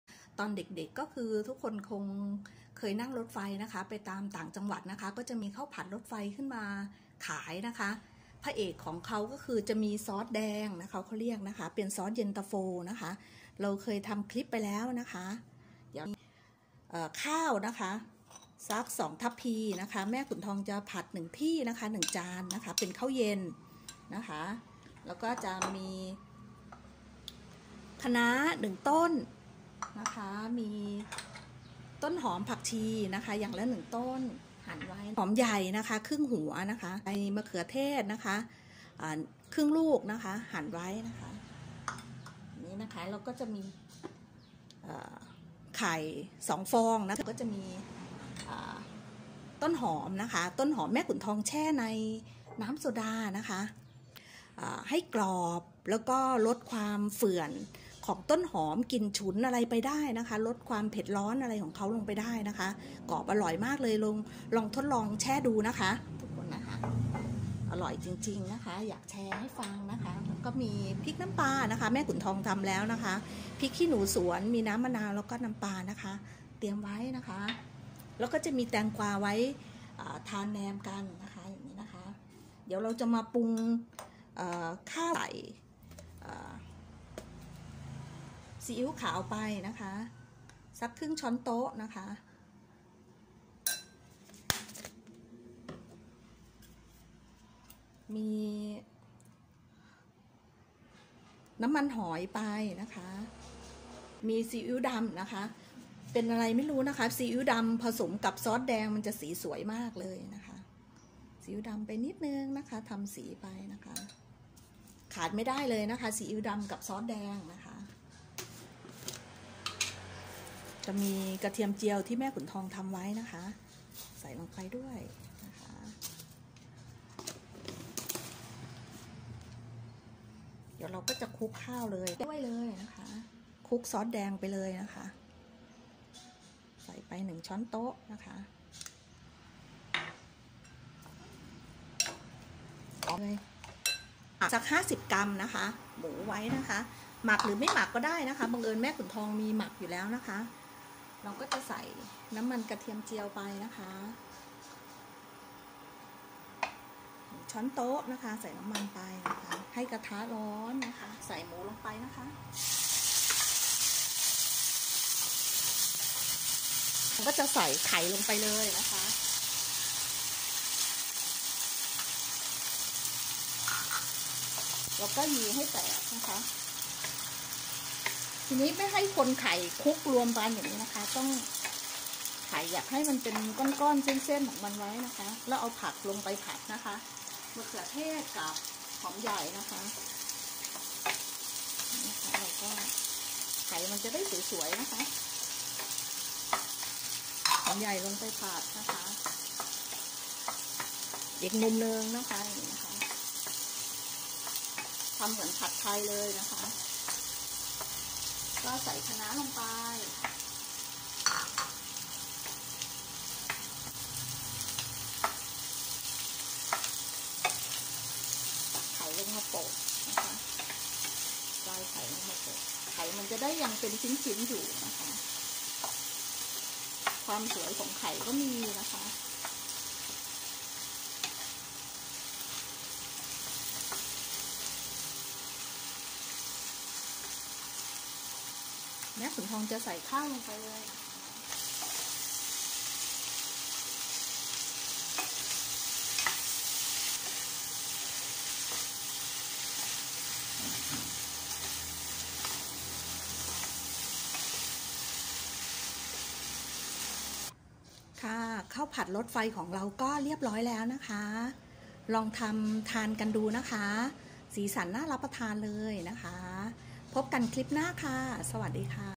@ข้าวผัดรถไฟ(ข้าวผัดซอสแดง)ย้อนวันวาน Railway Fried Rice (Red Sauce Fried Rice)
@ช่วง​เข้าครัว​กับ​แม่​ขุนทอง
@During the kitchen with Mae Khunthong
น้ำมันสำหรับผัดข้าว​ 1​ ช้อนโต๊ะ​
ข้าวสวยหุงสุกค้างคืน​ 2​ ทัพพี
เนื้อหมู​ 50​ กรัม
คะน้า​ 1​ ต้น
หัวหอมใหญ่​ 1/2​ หัว
มะเขือเทศ​ 1/2​ ลูก
ไข่ไก่​ 1-2​ ฟอง
ต้นหอมหั่นท่อน​ 1​ ต้น
ผักชีหั่นท่อน​ 1​ ต้น
*ปรุงรสข้าวก่อนนำไปผัด
ซีอิ๊วขาว​ 1/2​ ช้อนโต๊ะ​
กระเทียมเจียว​ 1​ ช้อนโต๊ะ​
ซีอิ๊วดำเล็กน้อย​
ซอสเย็นตาโฟ(ซอสแดง)​ 1 ช้อนโต๊ะ
*พริกน้ำปลา
พริกขี้หนูสวนซอย​ ​4​ เม็ด
น้ำมะนาว​ 1​/2 ช้อนโต๊ะ​
*ผักแนมทานกับข้าวผัด
แตงกวา
ผักชี
ต้นหอม(แนะนำให้ทดลองเอาต้นหอมแช่ในน้ำโซดาเย็นจัดจะมีรสชาติดีกรอบอร่อยมากลด​กลิ่นฉุนและช่วยให้รสชาติของต้นหอมอร่อยขึ้น)​
1 tablespoon oil Steamed
Rice, cooked overnight, 2 scoops
Pork 50 g
1 kale
1/2 large onion
1/2 tomato
1-2 eggs
1 scallion, chopped
Coriander, chopped 1 plant
* Season the rice before stir frying.
1/2 tablespoon soy sauce
1 tablespoons oyster sauce
1 tablespoon fried garlic
Lightly dark soy sauce
Yentafo Sauce (Red Sauce) 1 tbsp.
* Chili with fish sauce
4 chillies
1 tablespoon fish sauce
1/2 tablespoons lemon juice
* Vegetables served with fried rice cucumber
coriander
Spring onion (recommended to try soaking spring onion in cold soda water, it will taste good, crispy, very tasty, reduce the pungent smell and improve the taste of spring onion)

@ซอสเย็นตาโฟ(ซอสแดง)​รสชาติจัดจ้านทำได้สารพัดเมนูYentafo Sauce (Red Sauce)spicy&made various menus.

รบกวนติดตามช่องแม่ขุนทอง

*ขอบคุณที่ติดตามและให้กำลังใจขอให้ทุกท่านมีความสุข, สุขภาพแข็งแรง, ร่ำรวยทุกท่านค่ะ Thank you for following and encouraging everyone to be happy, healthy, prosperous, wealthy, everyone.